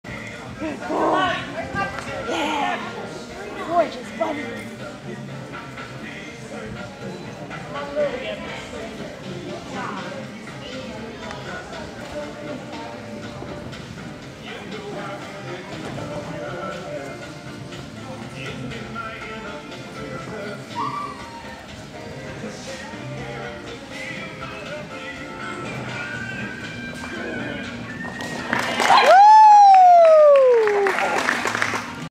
Cody Elite Tunnelers PZ July 2009 Sat
Wahoo!!! I LOVE running Tunnelers with my Cody-dude!!  And this was a fun course :-D